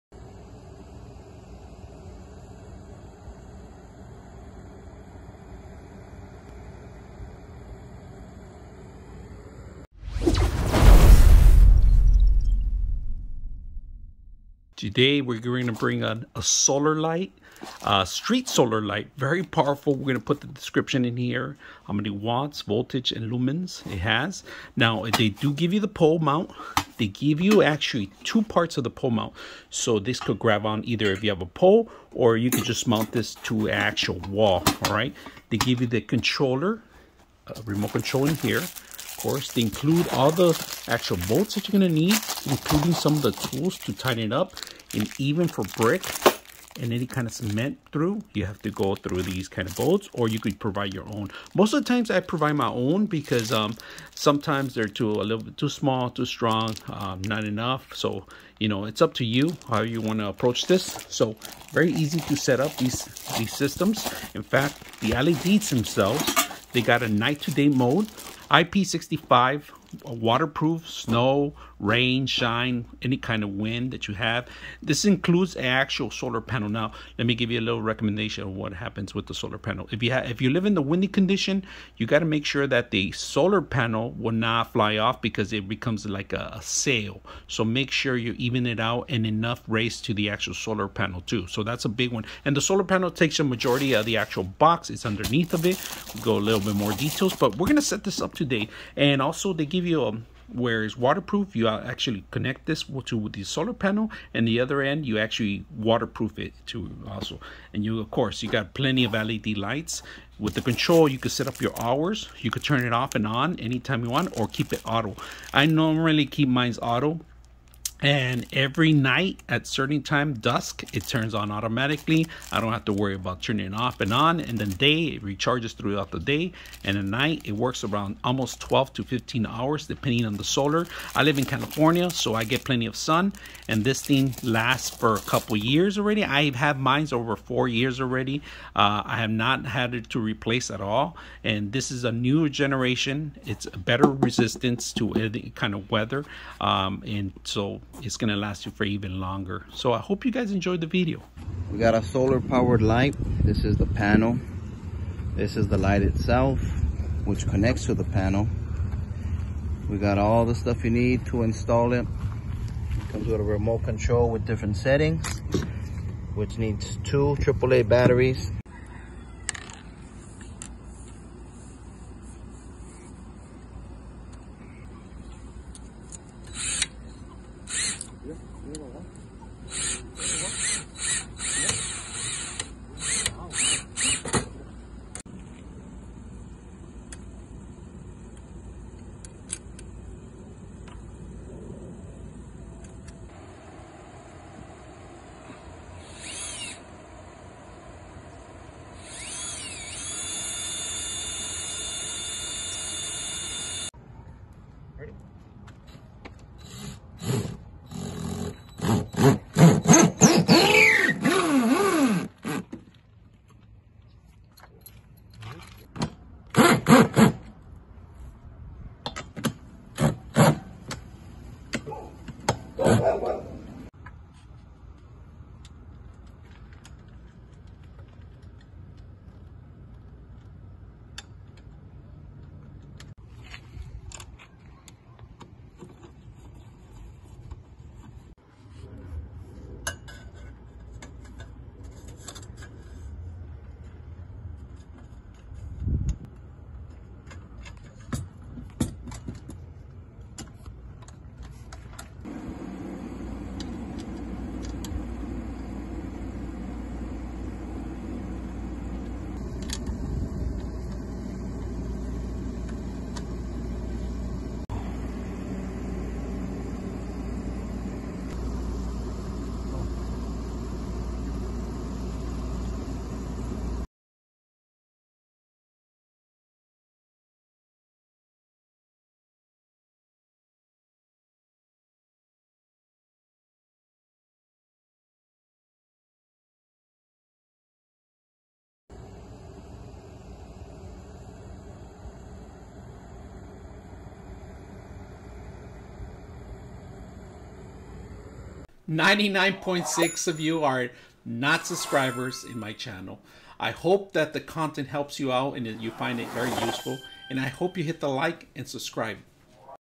500W Solar Street Lights Upgrade
A-ZONE 500W Solar Street Lights Outdoor, 50000LM High Brightness Dusk to Dawn LED Lamp, with Remote Control, IP65 Waterproof for Parking Lot, Yard, Garden, Patio, Stadium